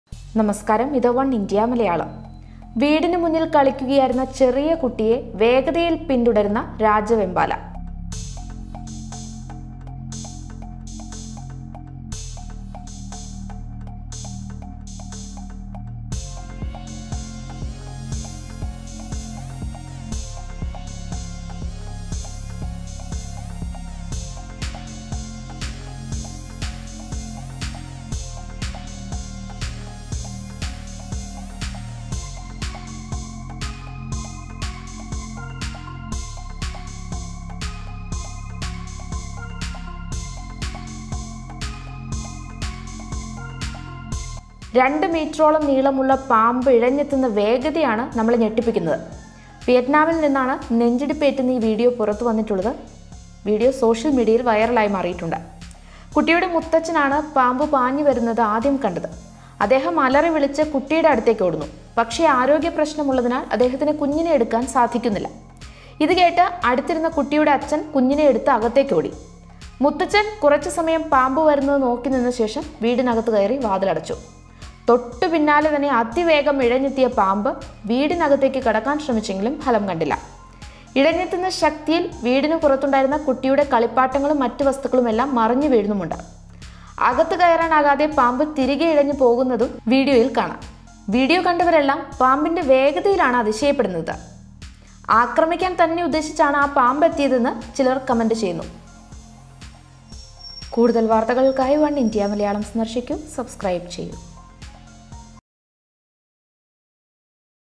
രാജവെമ്പാലയില്‍ നിന്ന് കുഞ്ഞു രക്ഷപ്പെട്ടത് തലനാരിഴക്ക്..| Oneindia Malayalam
King cobra tries to follow toddler inside house in Vietnam: video
കുഞ്ഞിനെ പിന്തുടര്‍ന്ന് കൊടുംവിഷുള്ള രാജവെമ്പാല. 2 മീറ്ററോളം നീളമുള്ള പാമ്പ് ഇഴഞ്ഞെത്തുന്ന വേഗതയാണ് നമ്മളെ ഞെട്ടിപ്പിക്കുന്നത്.കുഞ്ഞ് തലനാരിഴയ്ക്കാണ് രക്ഷപ്പെട്ടത്‌